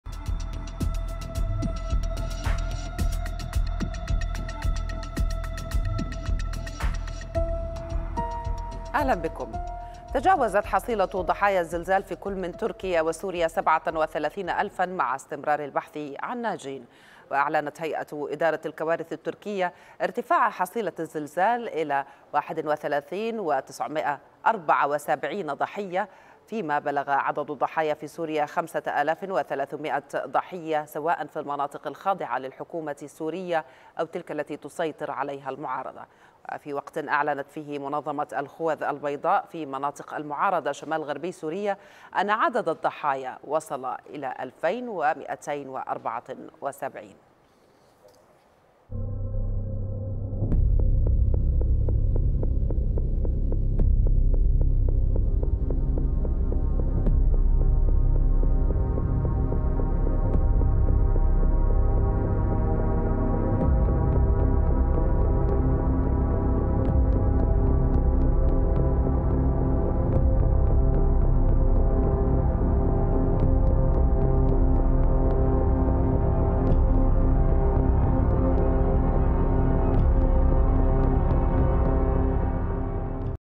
حصيلة ضحايا زلزال تركيا وسوريا تتجاوز الـ 37 ألف وفاة - أخبار الشرق
تجاوزة حصيلة الزلزال في كل من تركيا وسوريا 37 ألفا مع استمرار البحث عن ناجين، وأعلنت هيئة إدارة الكوارث والطوارئ، الثلاثاء، أن حصيلة ضحايا الزلزال المدمر في تركيا ارتفعت إلى 31،974 حالة وفاة.
وأفادت الهيئة بإجلاء 195 ألفاً و962 متضرراً من المنطقة التي دمرها الزلزال في جنوب تركيا.
فيما ارتفع عدد الضحايا في سوريا 5,300 وفاة سواء في المناطق الخاضعة للحكومة السورية أو التي تسيطر عليها المعارضة.
وفي وقت أعلنت فيه منظمة الخوذ البيضاء في مناطق المعارضة شمال غربي سوريا أن عدد الضحايا وصل إلى 2,274 وفاة.